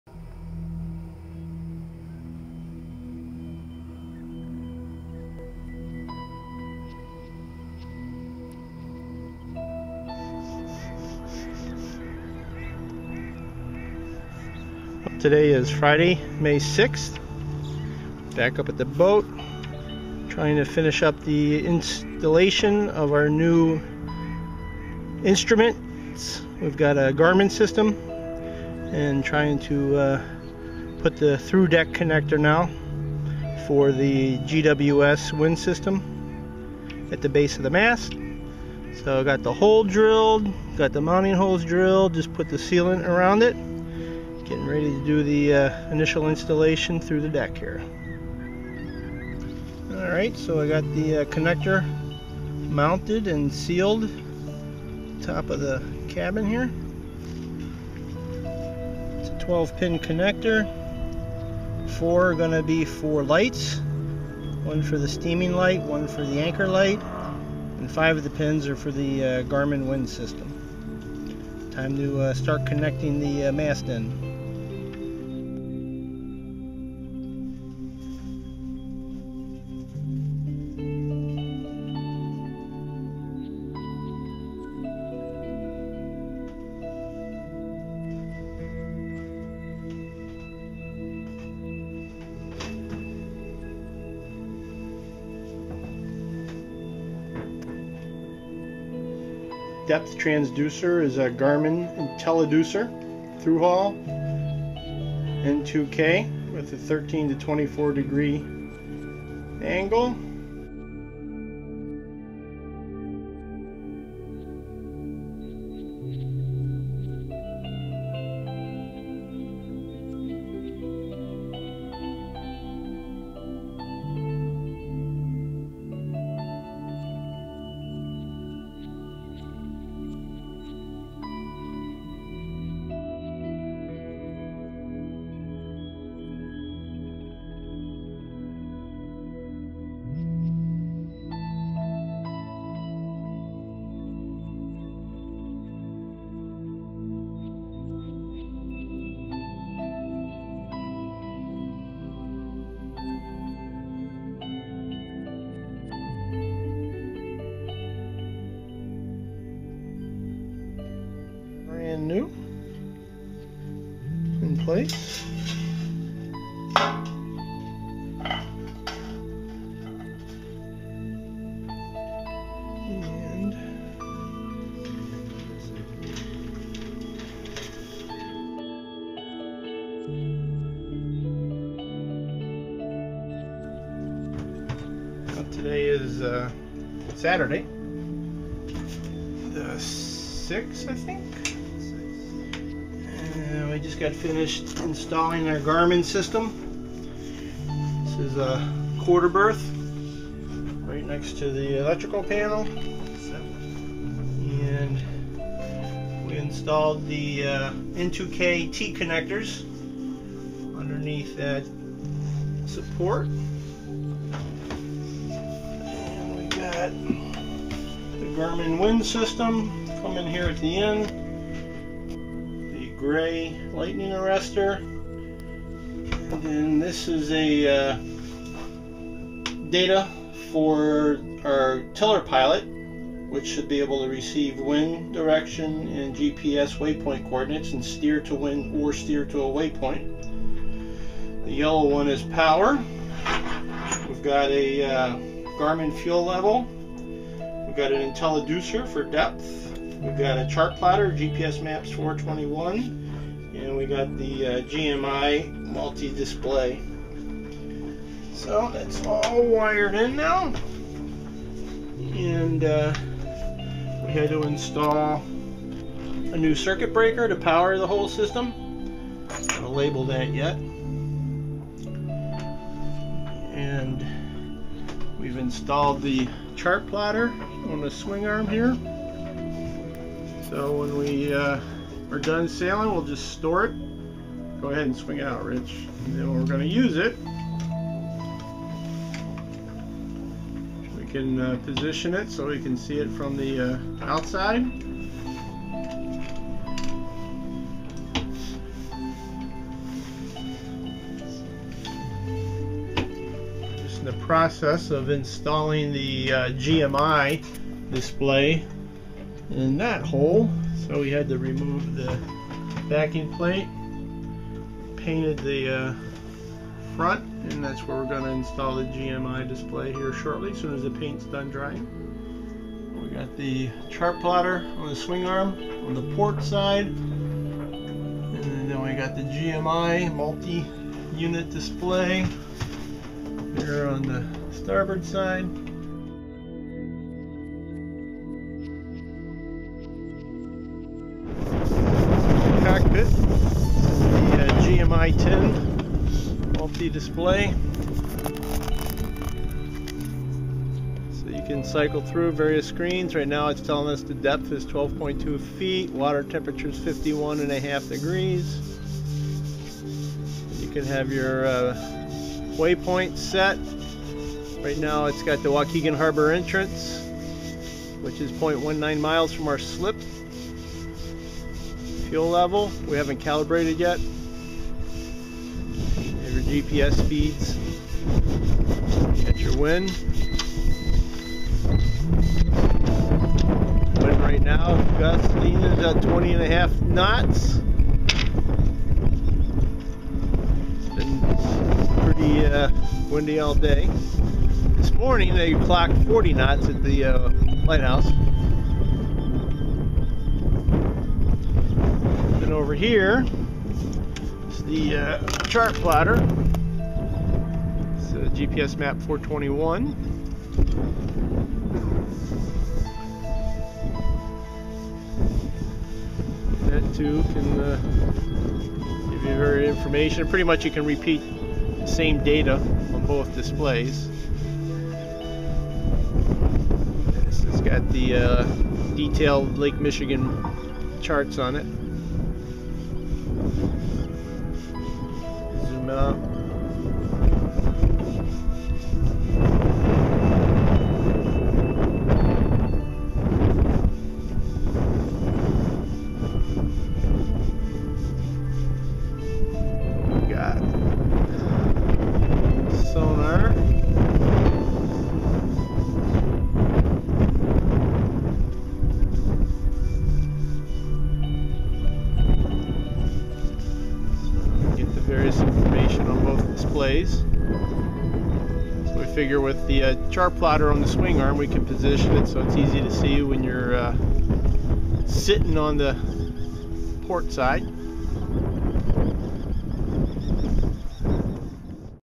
Sailing "Thanks Dad" Apr/May 2016 - Garmin NMEA 2000 Instrumentation Installation
GARMIN NMEA 2000 Instrumentation Installation.
Includes: GWS 10 Wind System, GFL Fuel Level, Intelliducer Depth, GMI-10 Multi-function Display & GPSMAP 421 Chartplotter

Over the winter we bought a new Garmin NMEA 2000 system.

The first piece was a Garmin GWS-10/GMI-10 system

While on the hard, we mounted a new masthead with our new Garmin NMEA 2000 GWS-10 wind system and dropped the NMEA 2000 cable down the mast.

After launch we installed a thru-deck connector at the base of the mast to bring the NMEA 2000 cable inside the boat.  It is a 12 pin connector.  5 pins for the NMEA 2000 cable and 4 for the mast lights.  The steaming light and the anchor light.

Also, while on the hard, we removed an old depth transducer and installed a new Garmin NMEA 2000 Intelliducer.  Using a level with an angle finder we found the location on the hull where the Intelliducer was to be mounted was on a 18 degree angle.  So we bought the Intelliducer suitable for use with a 13 to 24 degree range.

One of our other winter projects was to remove the fuel tank to give it a good cleaning.  While out, we decided to add a resistive sender unit (WEMA KUS SSL) to measure fuel level and used a Garmin NMEA 2000 GFL adapter to interface it NMEA 2000 system.

After reviewing our options, we decided to mount the NMEA 2000 backbone “tee” connectors under the starboard quarter berth.  They would be centrally located to the devices, close to the electrical panel and easily accessible.

We mounted the GMI-10 multi-functional display on the starboard side

We mounted the GPSMAP 421 on a swing arm.  When in use it swings out in the companionway, then for storage it folds up out of the way against the cabin wall and can be operated easily from inside the cabin.